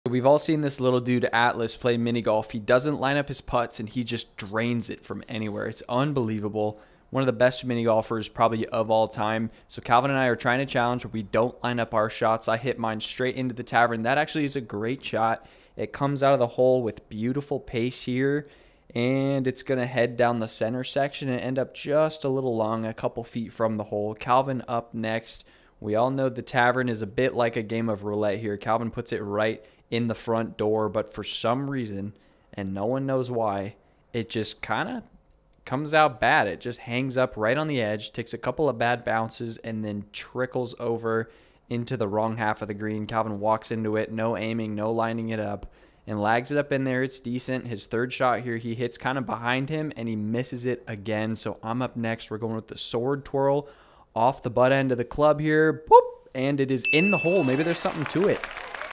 Mini Golf But You Can't Aim Your Putt!!
Are we as good at mini golf as this little guy @atlasdonyoung aka the best mini golfer ever??